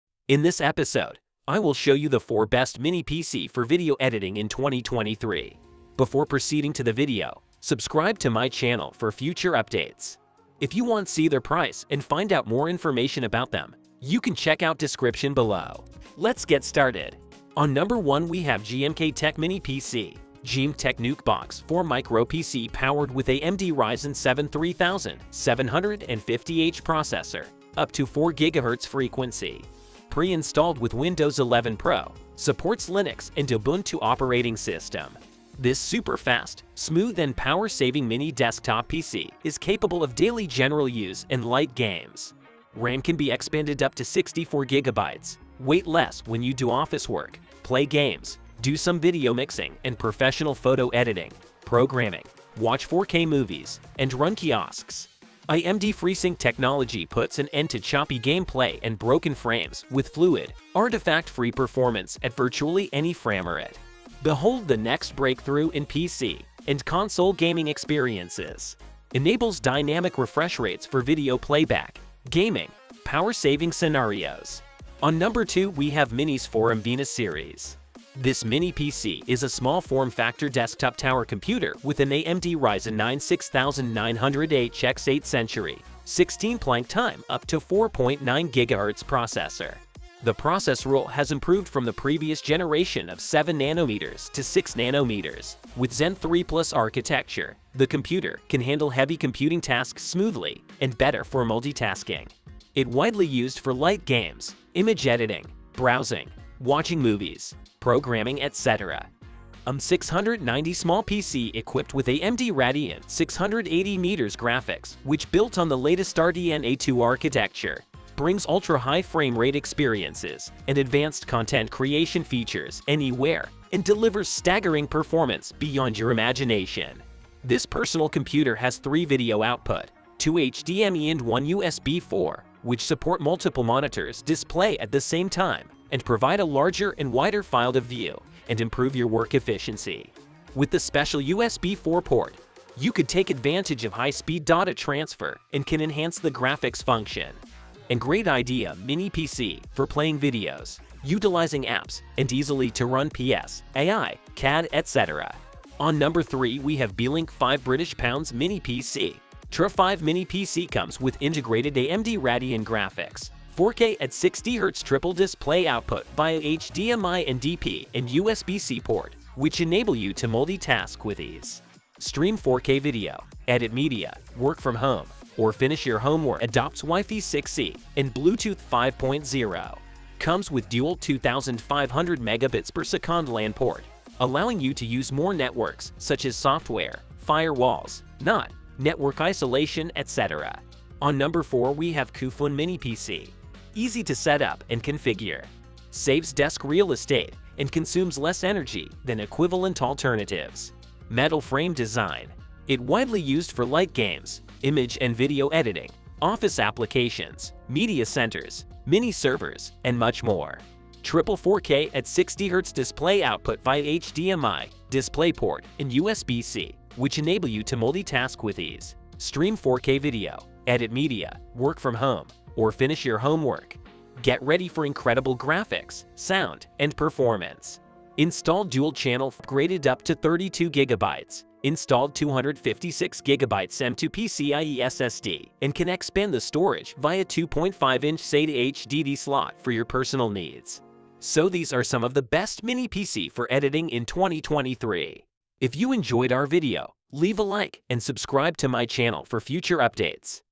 4 Best Mini PC for photo and video editing in 2023
List of 4 best Mini PC Computer for Video and photo editing to buy in 2023.

1. GMKtec Mini PC for editing video and photo

2. Minisforum Venus series Mini Computer for editing

3. Beelink GTR5 mini PC for Video editing and photo editing

4. Coofun photo and video editing mini PC